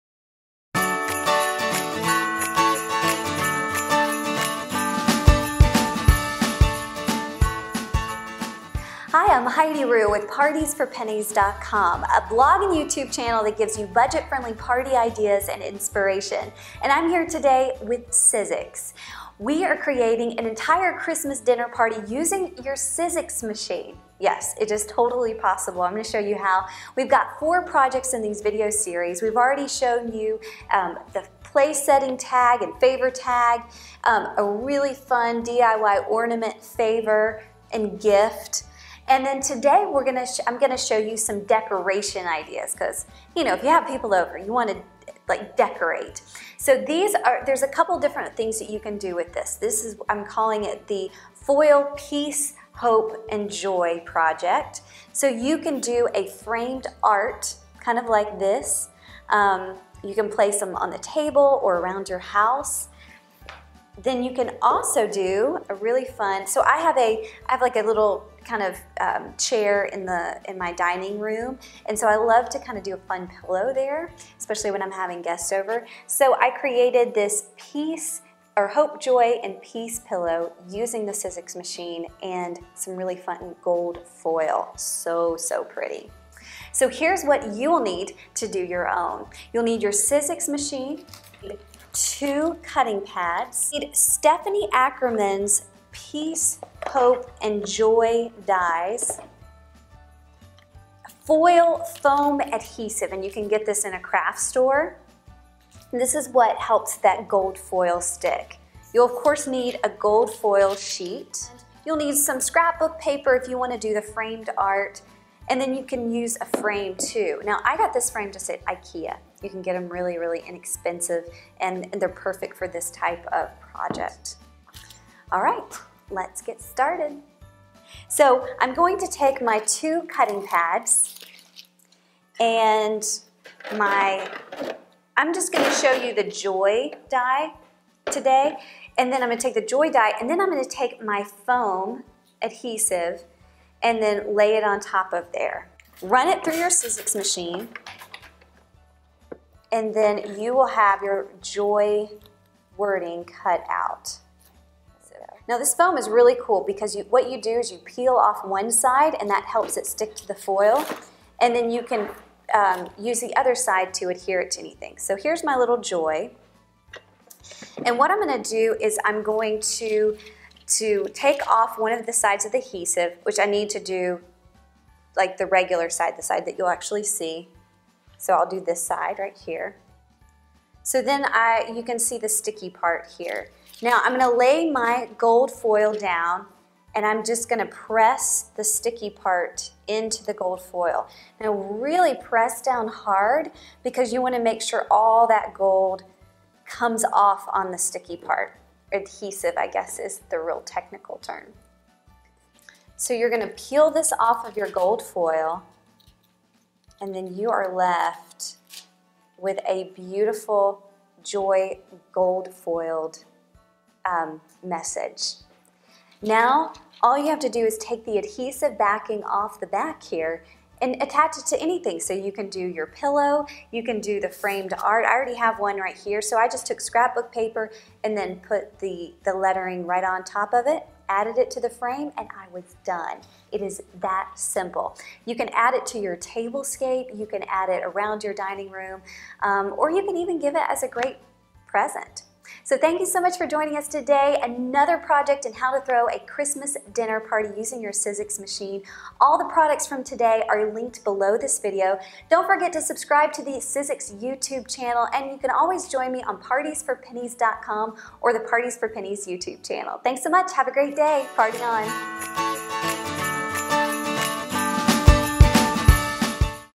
DIY Christmas Dinner Party: Foil Peace, Hope and Joy Project
Christmas is quickly approaching and Heidi Rew is back again to show us how simple it is to make a wonderful Christmas dinner party with the Big Shot Machine. In part 3 of this 4-part series, we learn how to make home décor pieces using gold foil sheets, perfect for using on your table or on pillows for your living or dining room. Let’s get Christmas crafting!